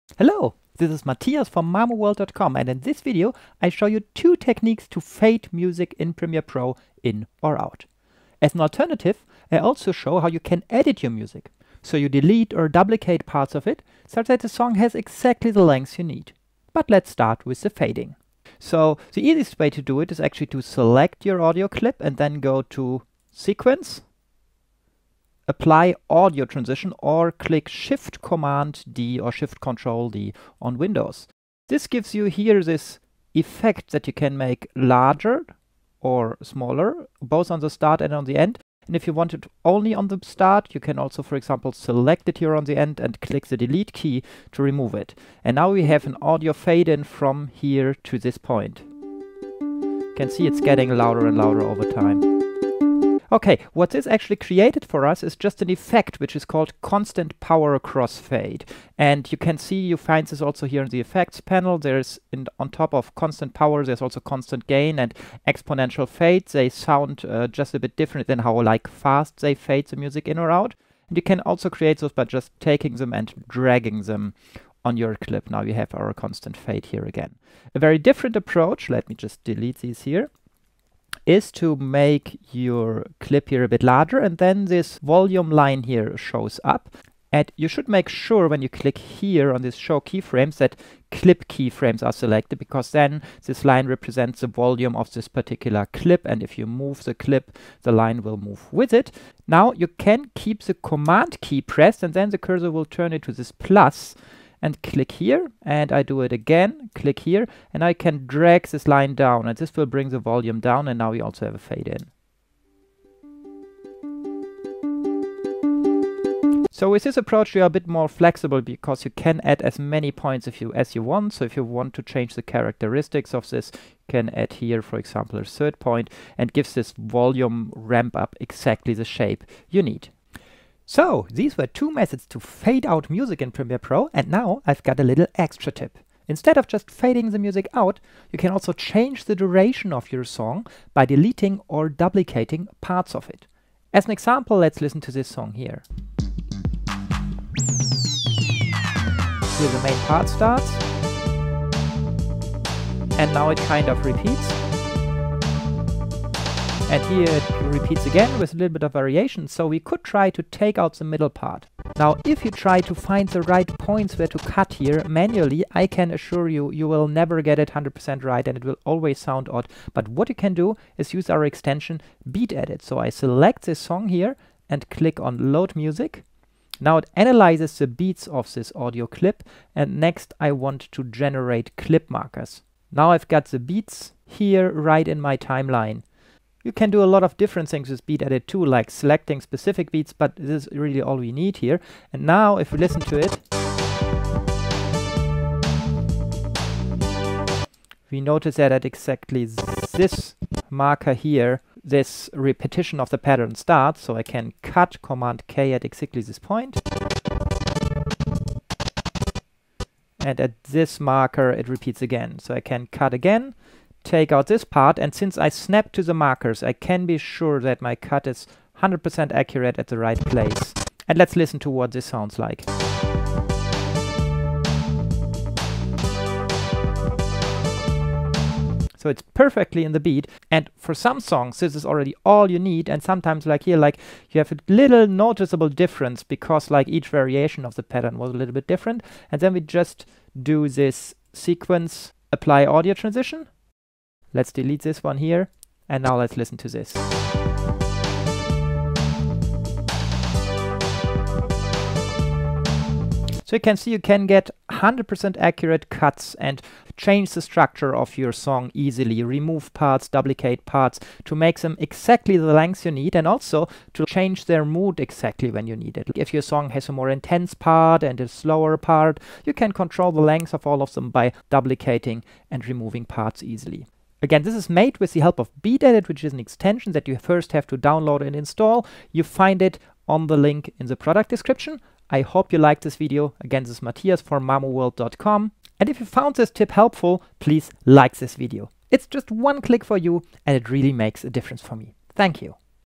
How to Fade Out Music in Premiere Pro CC 2020 - Quick Tip
Learn how to fade music in and out with Adobe Premiere Pro. I show you two techniques to do that: Either with clip keyframes or with a cross fade transition (Constant Power, Constant Gain, or Exponential Fade). In addition, I show you an alternative to just fading the sound out: You can also edit the song and rearrange, duplicate or delete parts such that it has exactly the length you need.